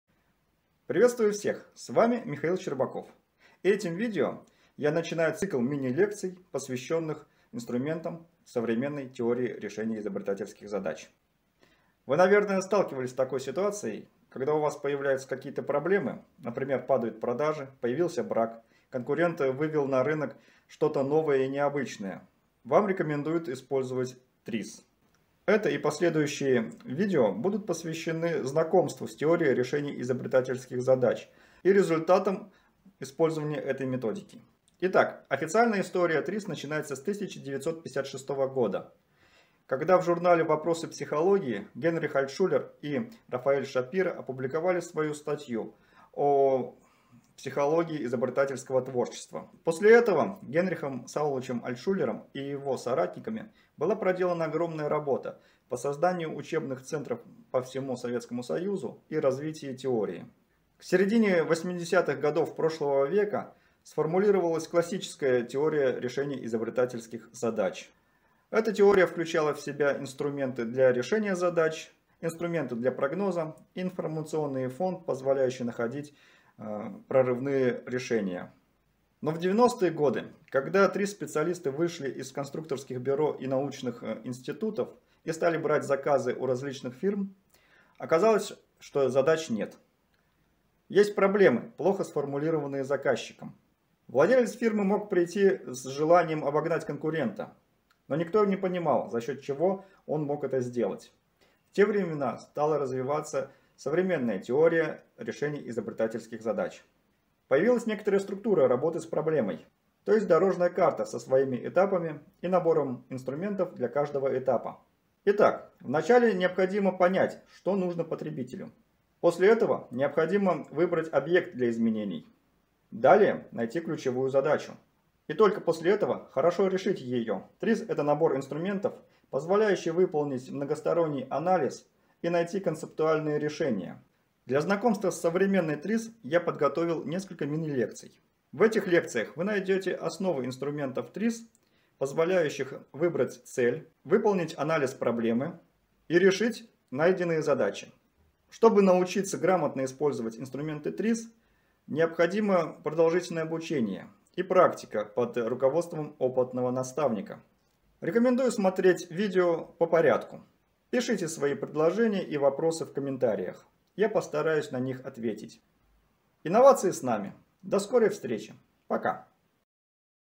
ТРИЗ кратко. Вступление
Видео, открывающее цикл мини-лекций по инструментам современной ТРИЗ.

0:00 Вступление
0:12 Проблемы и рекомендация
0:40 История ТРИЗ. Классическая ТРИЗ
1:20 Современная ТРИЗ
1:53 Дорожная карта ТРИЗ-проекта